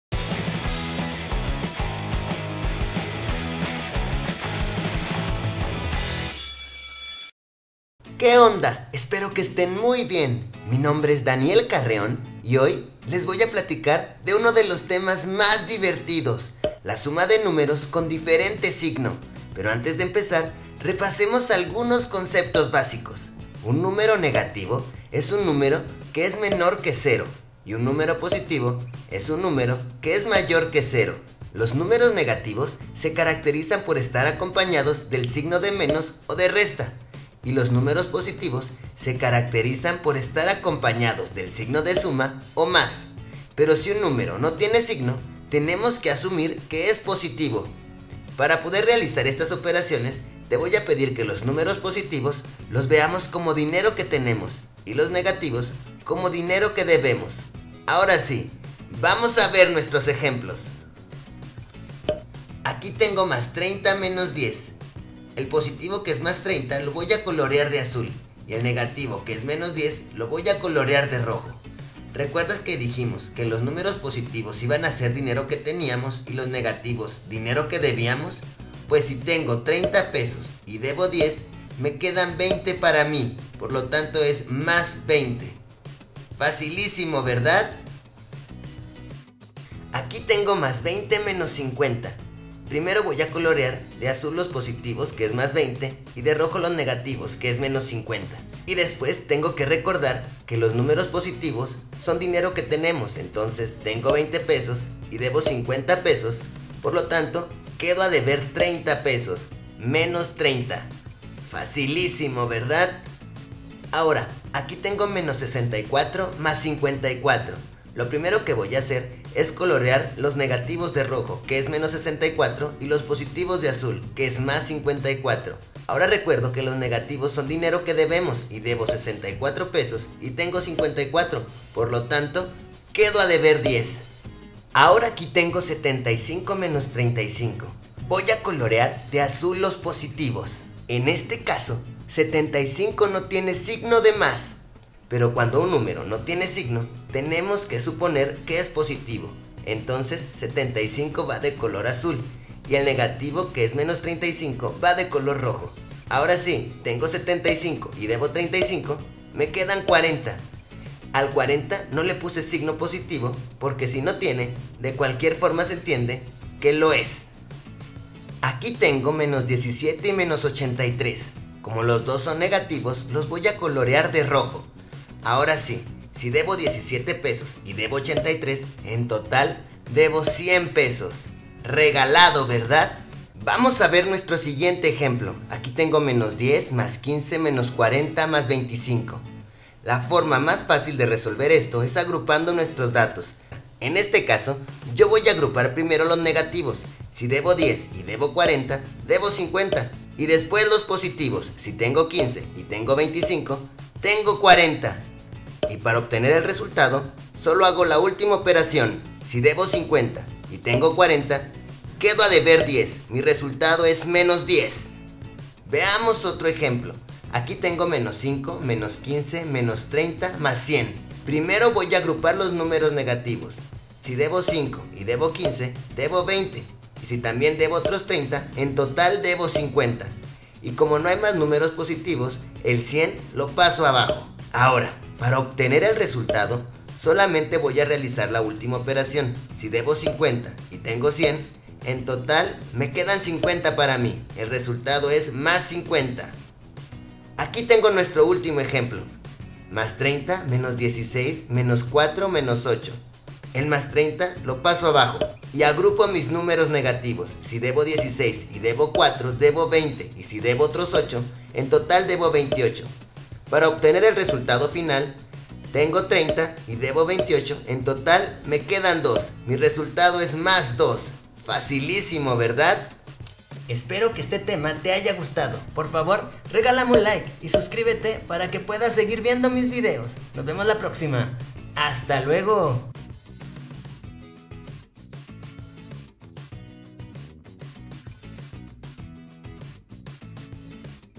SUMA DE NUMEROS CON DIFERENTE SIGNO Super facil
Hola, les comparto este video que explica la suma de numeros con diferente signo, espero les guste y me entiendan, saludos!!!

0:00 Bienvenida
0:23 Conceptos basicos
1:02 Ejercicio 1
1:30 Ejercicio 2
1:54 Ejercicio 3
2:15 Ejercicio 4
2:48 Ejercicio 5
3:06 Ejercicio 6
3:37 Ejercicio 7
4:10 Ejercicio 8